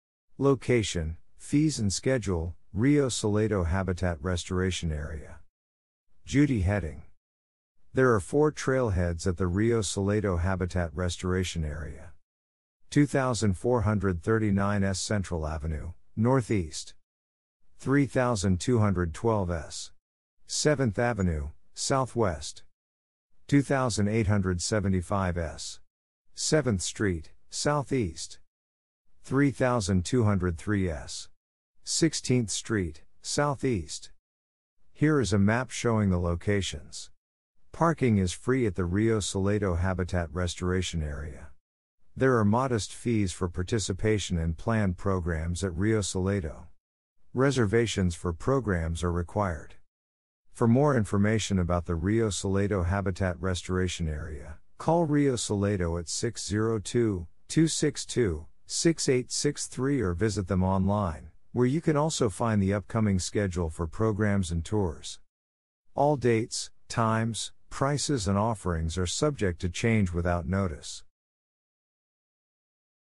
Location Fees and Schedule
Judy Hedding.
There are four trailheads at the Rio Salado Habitat Restoration Area:.
- 2439 S. Central Avenue (northeast).
- 3212 S. 7th Avenue (southwest).
- 2875 S. 7th Street (southeast).
- 3203 S. 16th Street (southeast).
Here is a map showing the locations. Parking is free at the Rio Salado Habitat Restoration Area. There are modest fees for participation in planned programs at Rio Salado. Reservations for programs are required..
For more information about the Rio Salado Habitat Restoration Area, call Rio Salado at or visit them online, where you can also find the upcoming schedule for programs and tours..
All dates, times, prices and offerings are subject to change without notice..